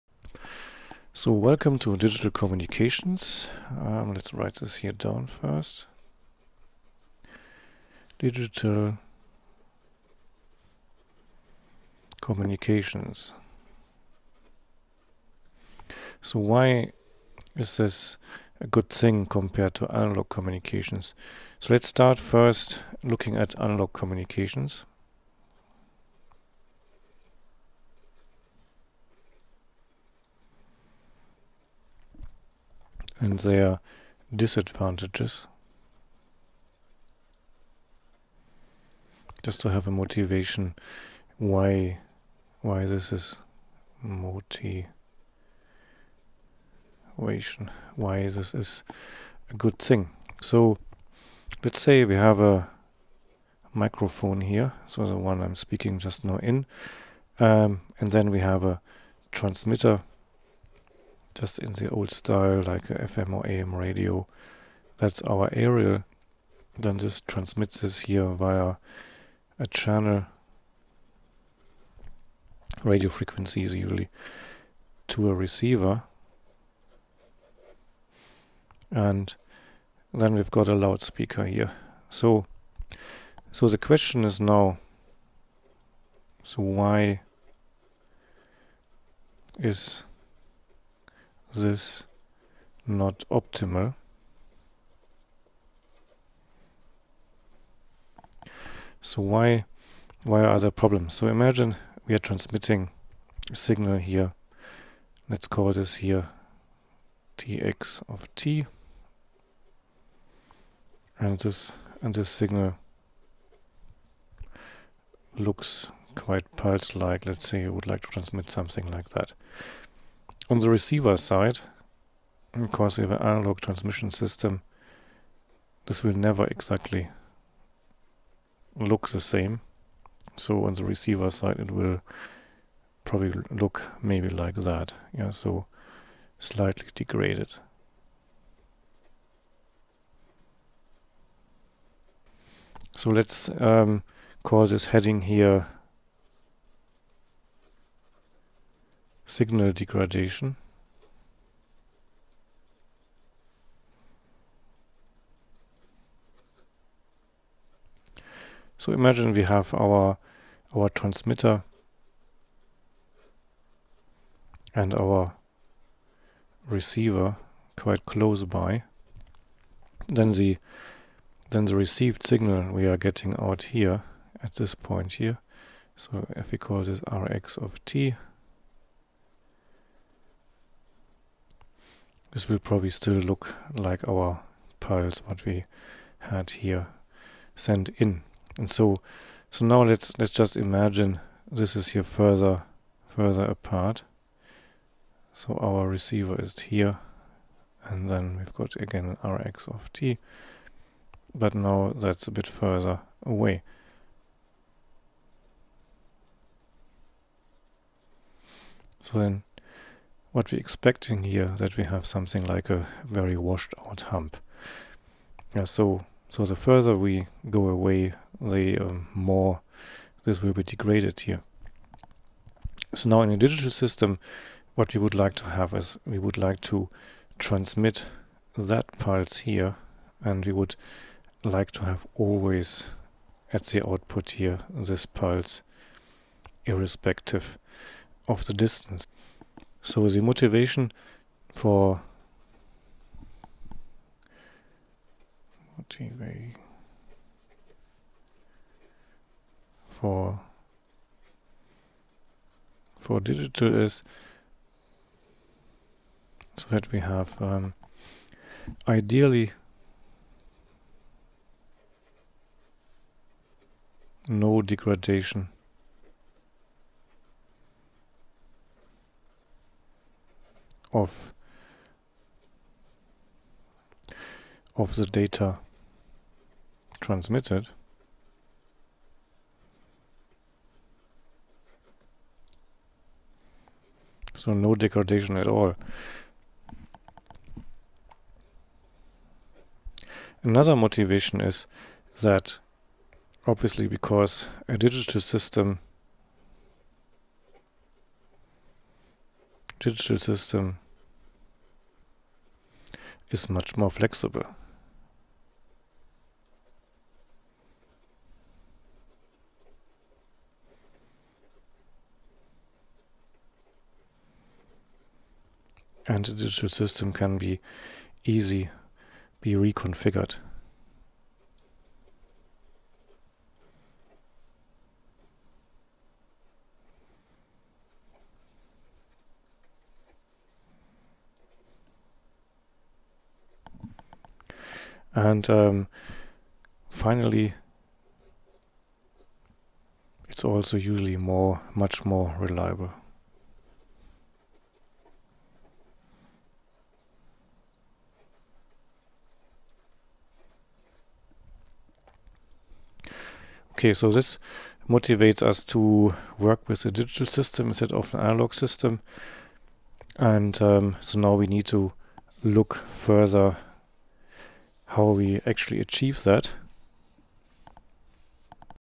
Introduction: Why do we need digital transmission? (#0000)
Analgoue transmission slowly degrades the signal whereas digital tranmission can keep the signal intact.